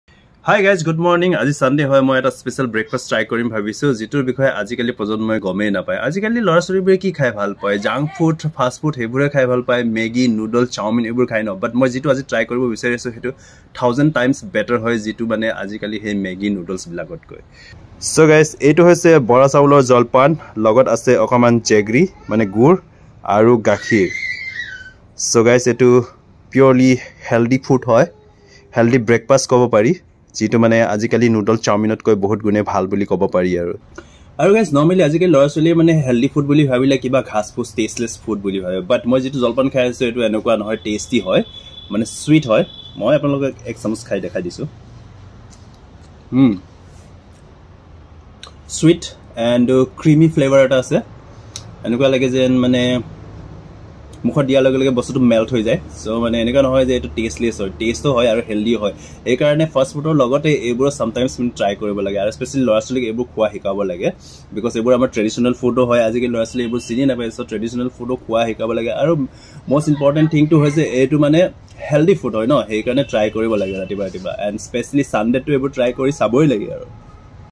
কেনেকৈ healthy breakfast খাব পাৰি?
As a matter of fact, junk food and fast food have become part of our daily food menu. The gen z kids don't even know about our healthy tasty traditional breakfast. In this video I am having one of our traditional foods the "Bora saulr jalpan"